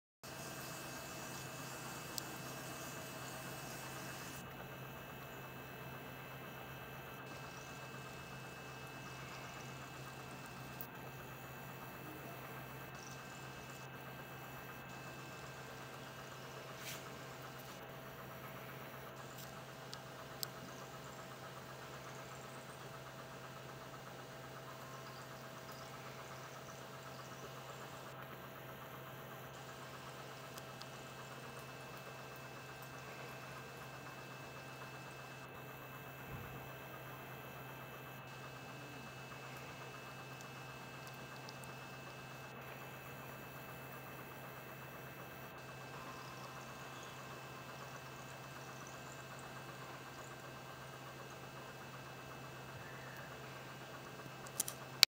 Olympus XZ-1 Camera 720p HD Video Macro Close-Up Auto Focus & Zoom Test Footage Demo
Bubbling water bottle test footage of the new Olympus XZ-1 Camera under indoor low lighting conditions and stressing it's auto focus + zoom capabilities.